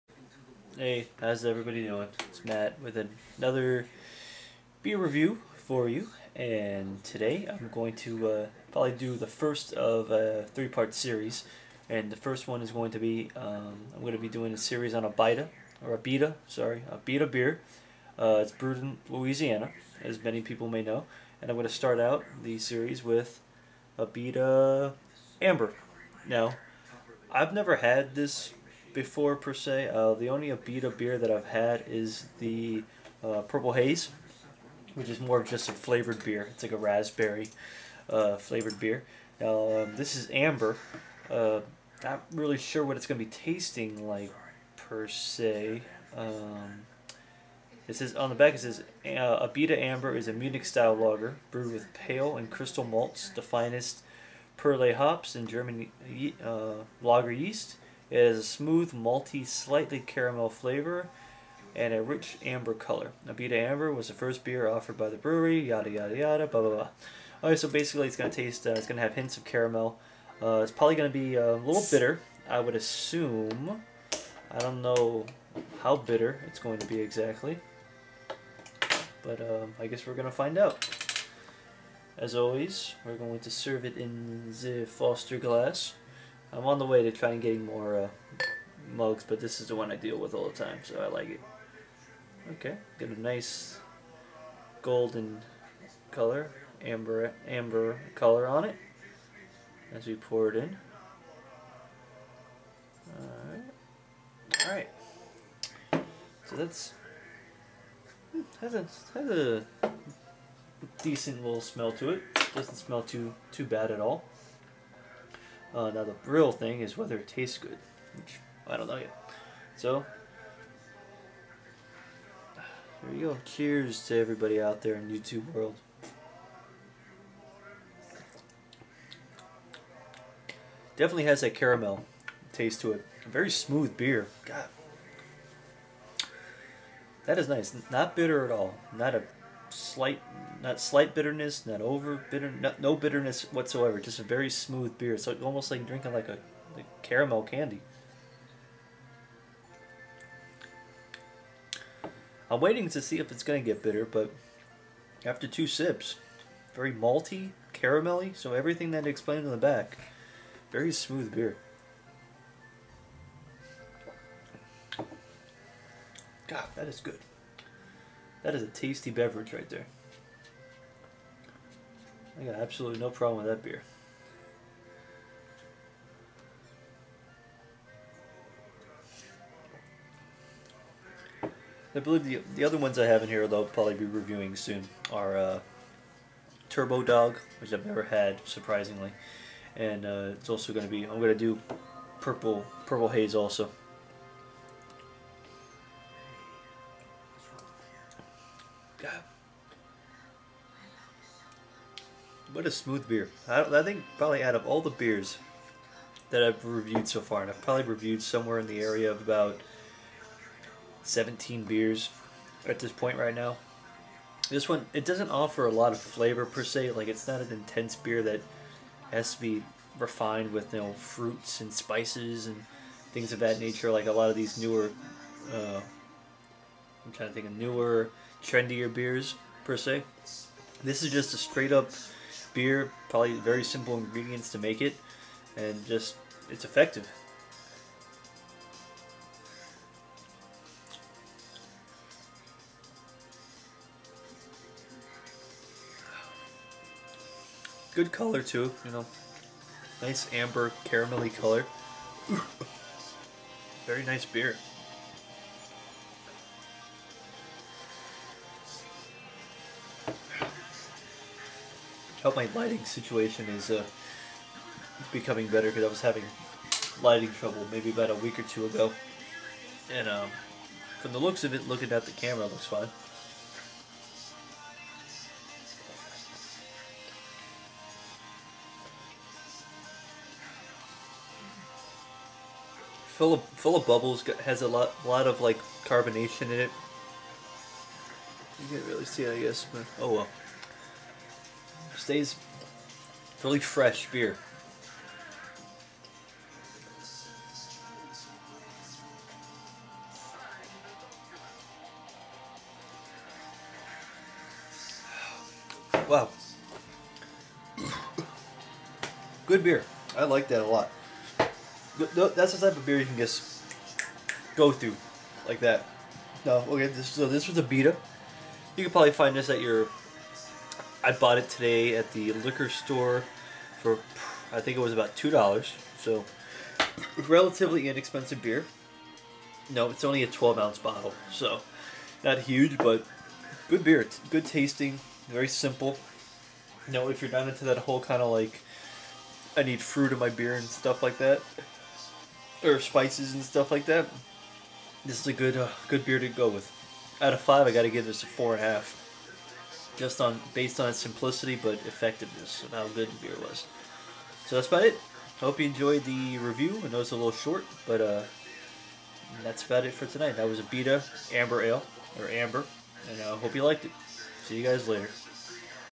Let's Drink: Abita Amber
I drink a frosty brew of Abita Amber.